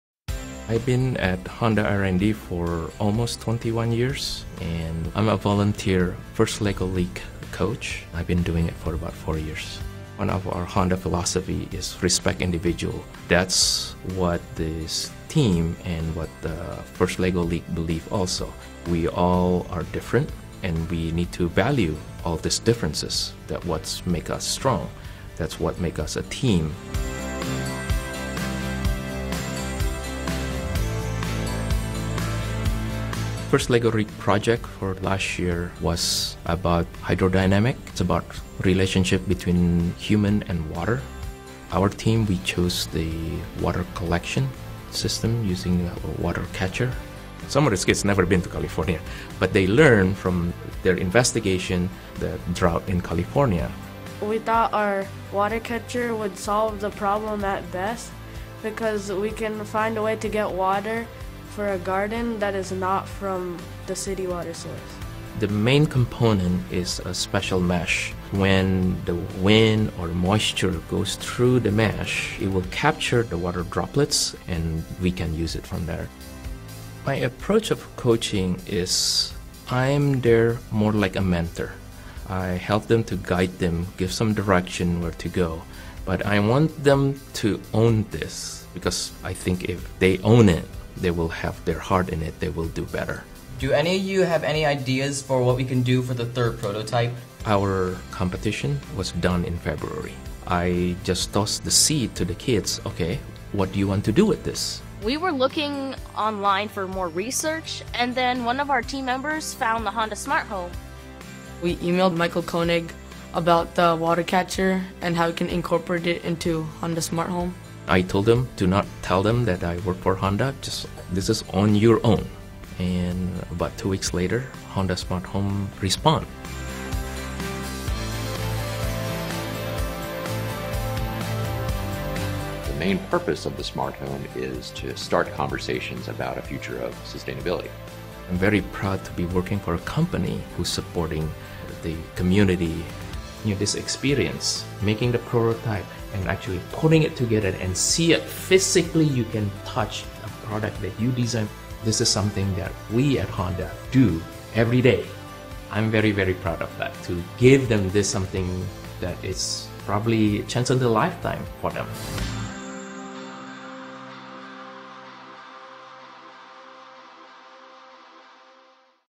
Honda R&D Americas Engineer Helps Students Develop New Technology
Ray Khouw, an engineer at Honda R&D Americas, Inc., lights up when he talks about the joy of taking an idea for a product from the initial design stage to mass production. He is even more passionate about sharing these engineering skills with the students from Team Techno Storm that he coaches in the FIRST LEGO League team.